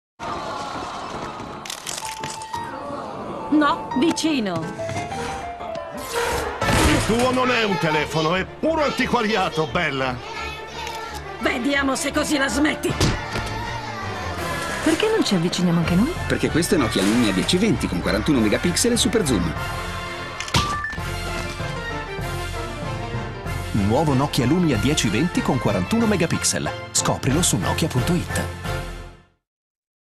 Nokia Lumia 1020 2013
Telecomunicato Nokia Lumia 1020 con 41 megapixel
Voce pubblicità: Dario Maria Dossena

Immagini musica personaggi e marchi presenti in questo video appartengono ai legittimi proprietari e sono qui riprodotti per fini puramente dimostrativi.